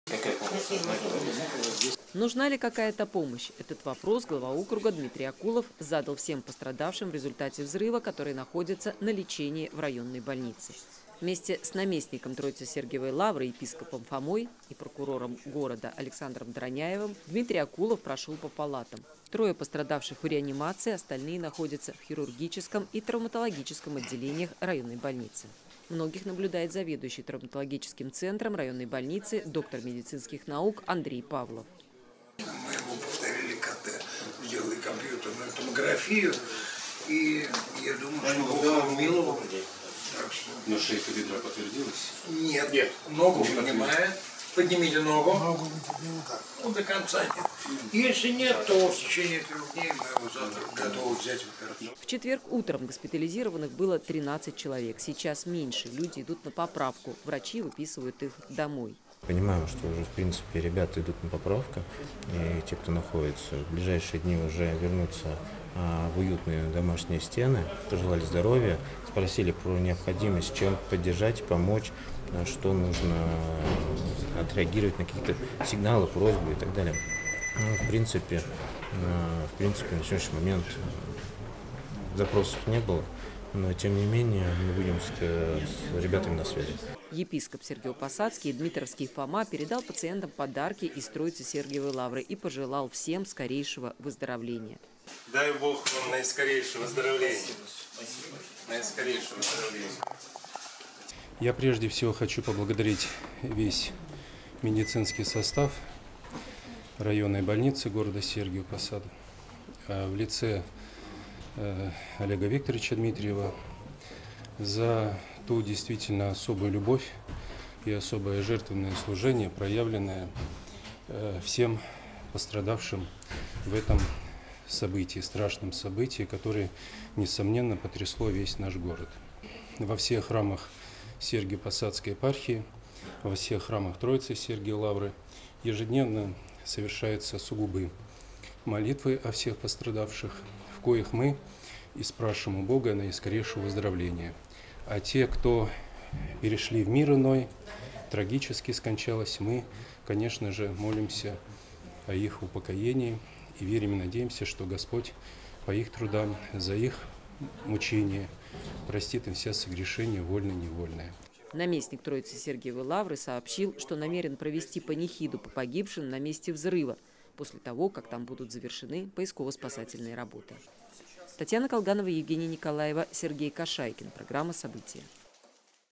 Скорейшего выздоровления!
Глава округа Дмитрий Акулов и наместник Троице-Сергиевой Лавры епископ Фома навестили пострадавших в РБ.